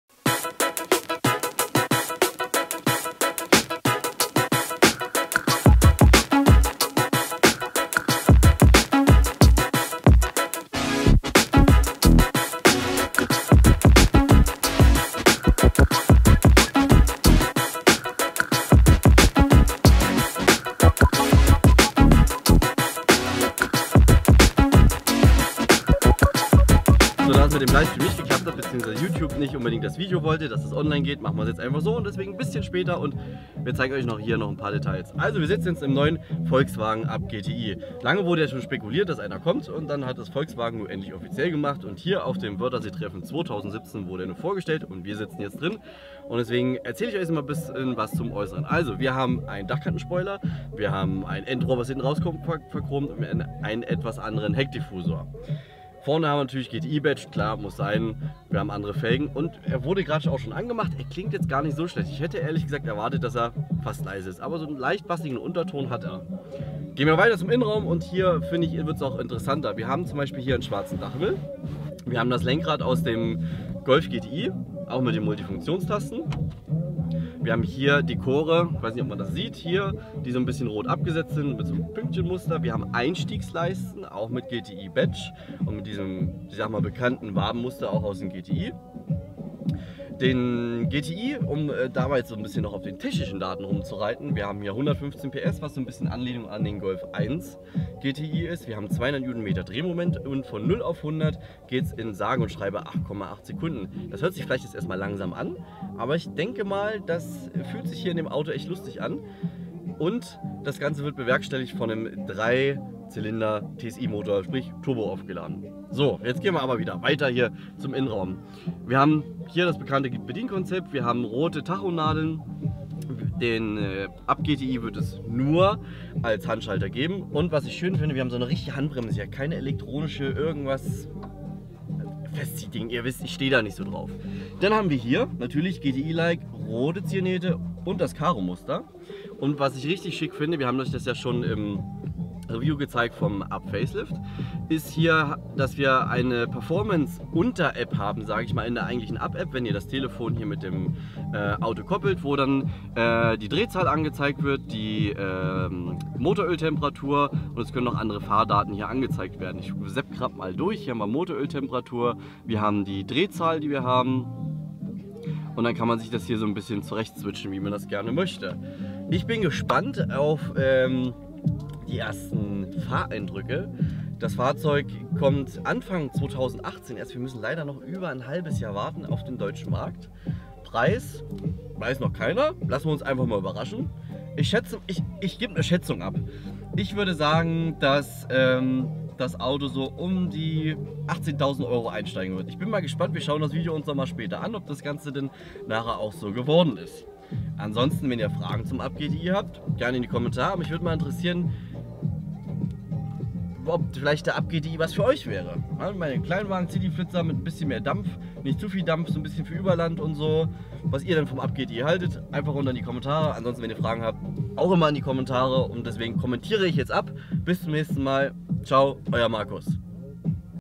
Das ist der NEUE VW Up! GTI | Wörthersee 2017 Volkswagen Release /// Lets Drive ///
Das ist er der neue 2018 Volkswagen Up GTI. Marcus hat ihn sich schon ein mal für euch genauer angeschaut.